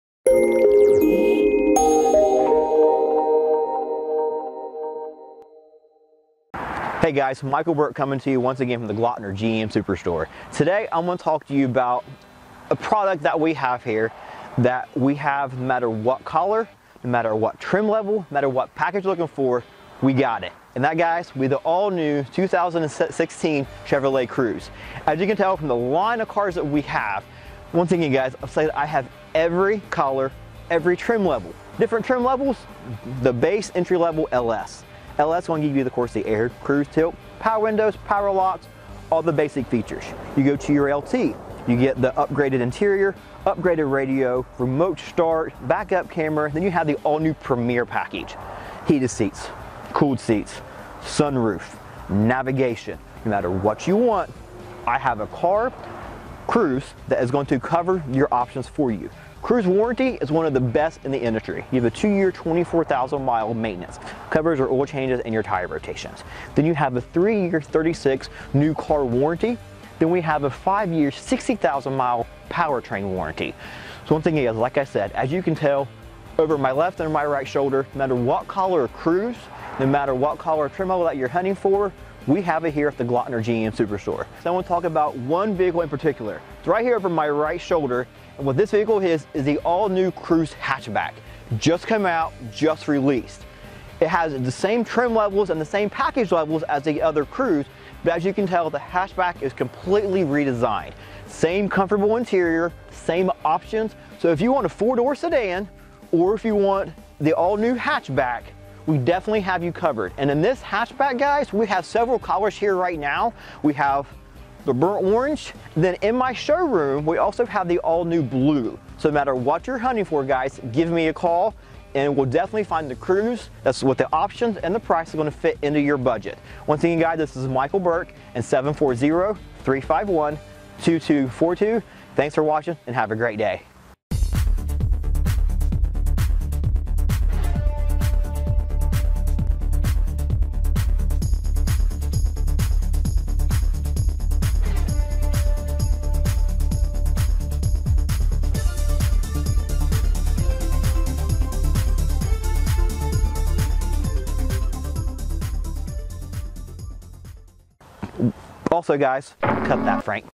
Michael and the 2017 Chevy Cruze Hatchback
Michael Burk of the Glockner GM Superstore shows you the all new 2017 Chevrolet Cruze Hatchback.  Stop in or visit us at Glockner.com where we make ownership easy!